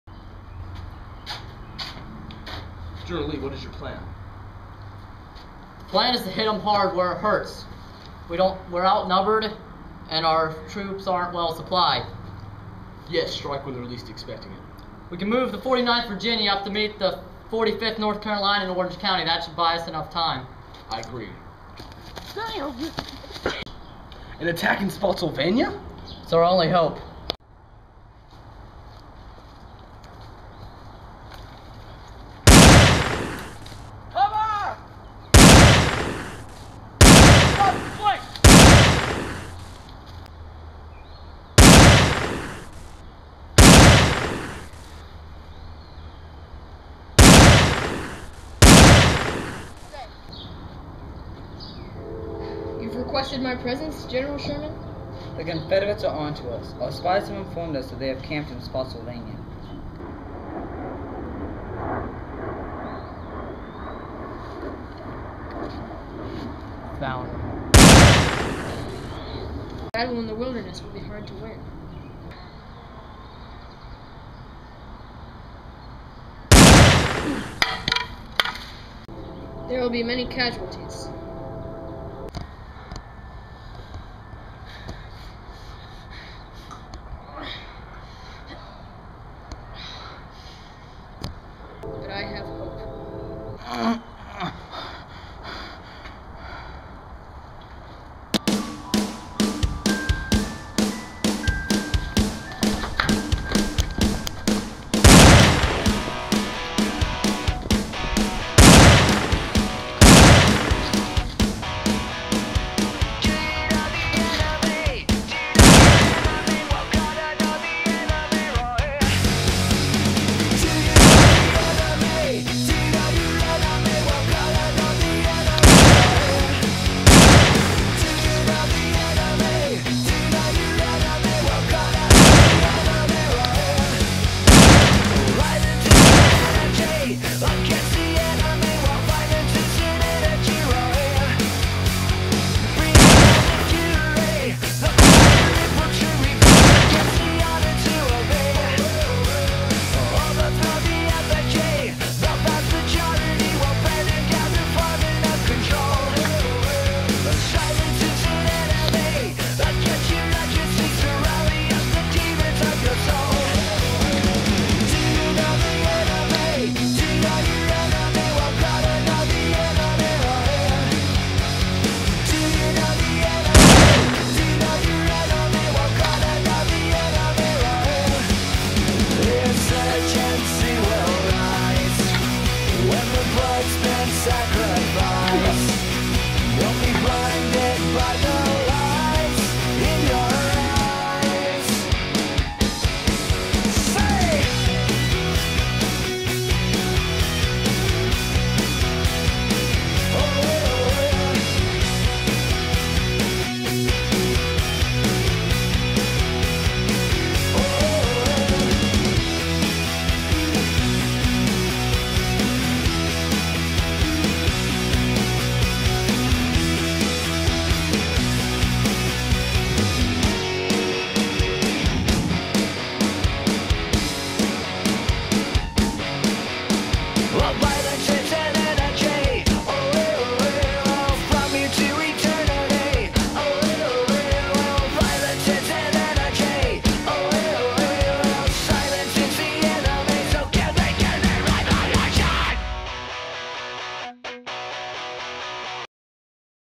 The Wilderness
Another video for Mr. Hooper's class. This was a lot of fun to make. Its kinda supposed to be the battle of the wilderness.